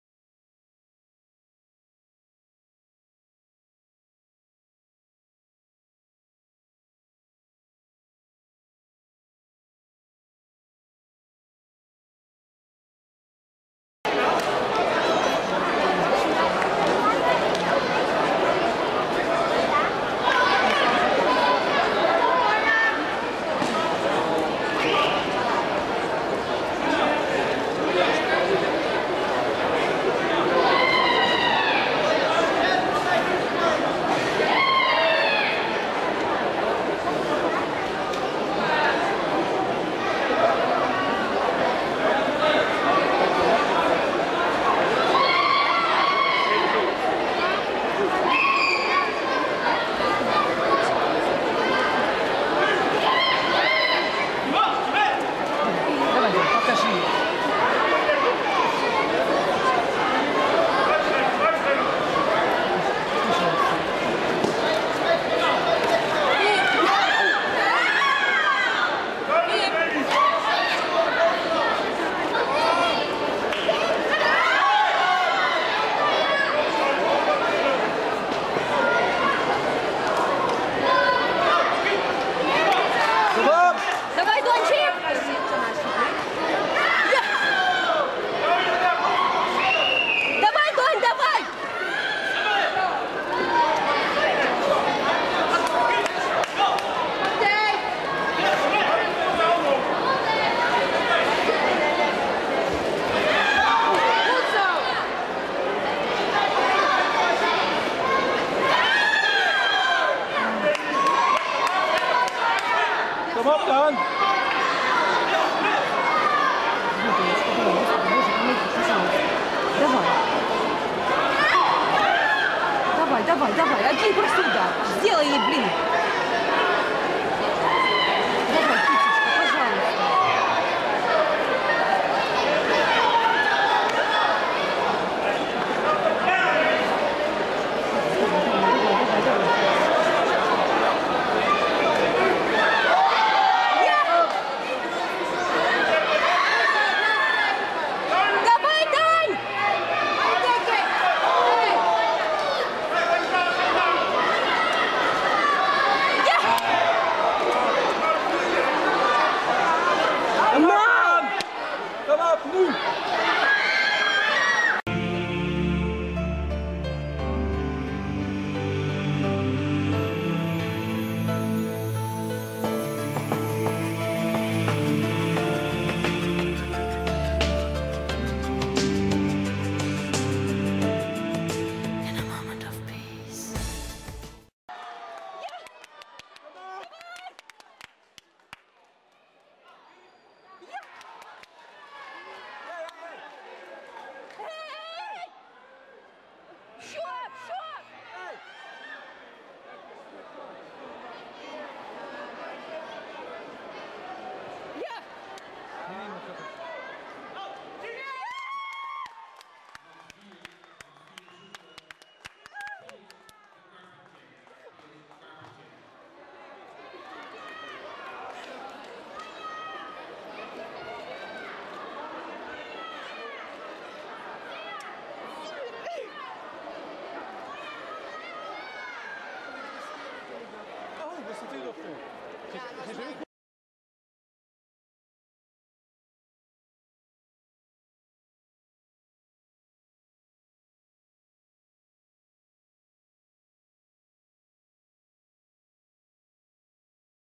NK-Jeugd-Karate 2011 Tanya Izelaar-(Unity'99) , Zoetermeer. Dutch Championship Karate Final 2011
NK Jeugd Karate 2011 Meisjes 10/11 jaar +40 Kg.
Tanya wint de Finale met een achterwaartse trap 4-2 !. Unity'99
Championship Karate 2011 Youth, The Netherlands. Tanya wins with a backward kick.